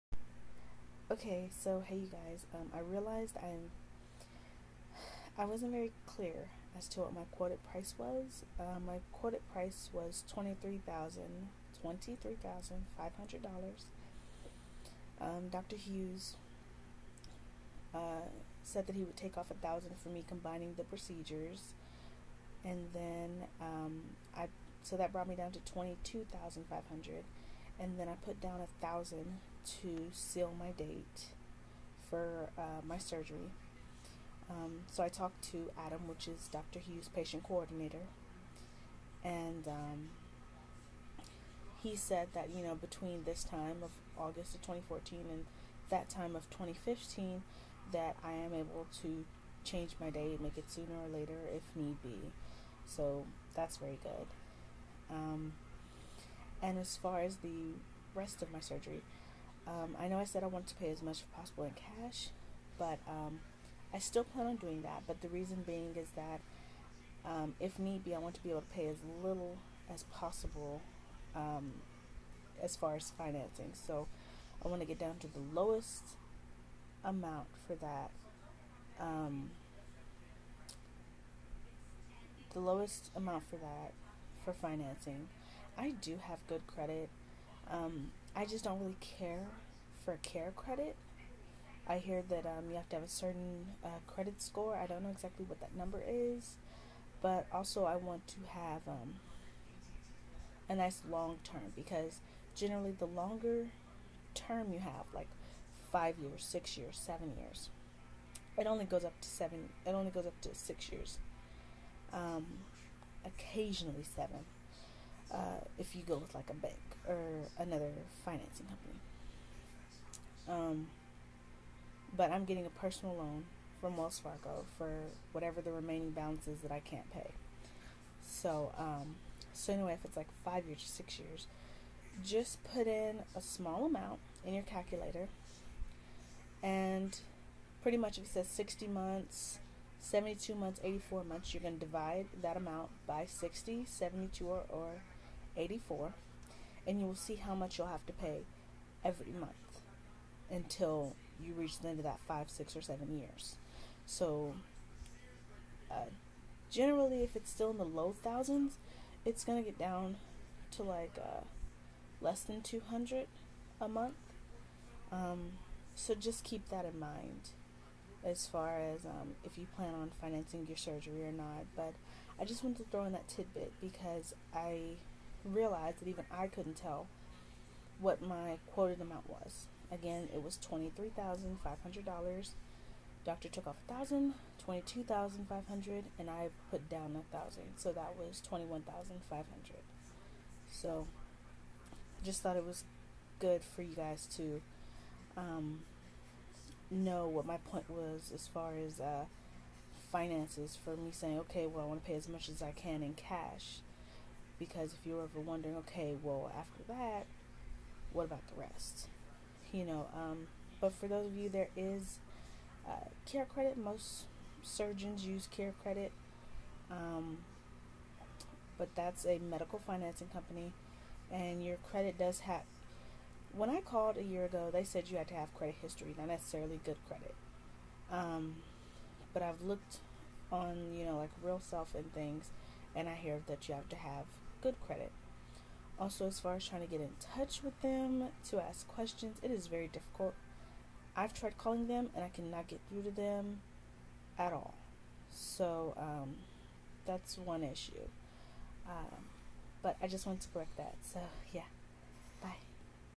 Correction to Consultation Video
This video is to correct any mistakes and clarify what i said in my consultation video.

This is the Website to research your surgery on if you are considering getting anything done you can find it all on here from follicle to cuticle:

This is the link to see my surgeons' YouTube channel to see him perform a few surgeries live:

NOTE: That there is NUDITY as in PICTURES in the story and on the site, that i will also post a link to, so please be mature and KEEP any NEGATIVE comments TO YOURSELF.